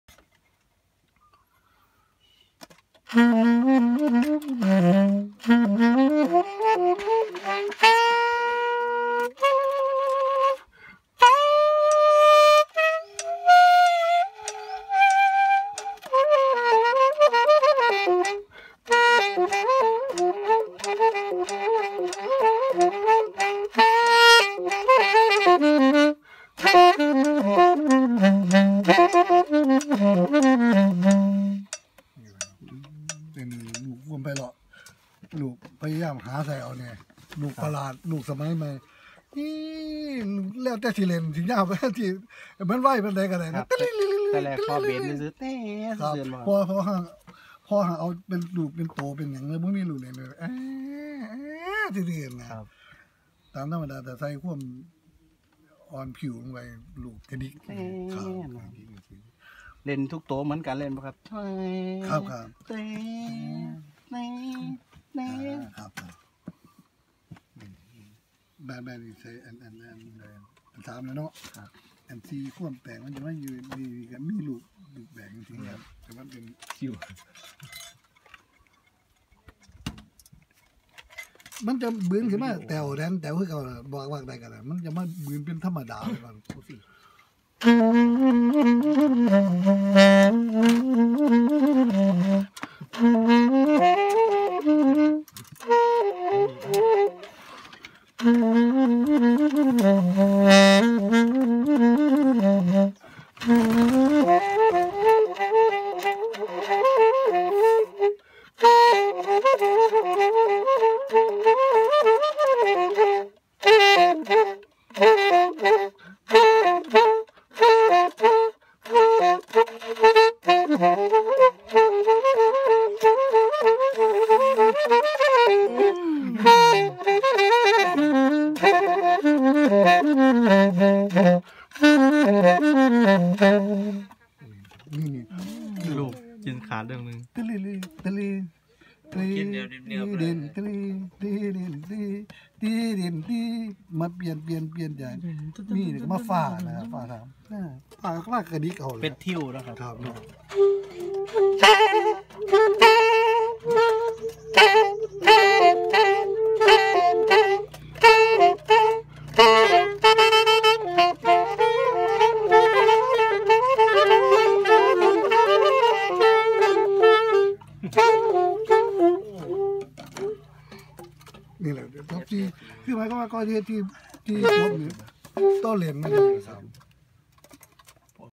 แซ็กโซโฟนอีสาน อ.สำราญ บุปผาวาสน์ (เว้าจาภาษาแซ็ก)EP-1
อ.สำราญ บุปผาวาสน์ ผู้นำแซ็กโซโฟน มาปรุยุกต์ใช้ในวงหมอลำคนแรก
ถ่ายทำที่ บ้านสร้างมิ่ง อ.ม่วงสามสิบ จ.อุบลราชธานี
โดยคณะครู และ นศ.สาขาวิชาดนตรี มหาวิทยาลัยราชภัฎอุบลฯ
F.เอเย่นต์ แซ็กโซโฟน อุบลราชธานี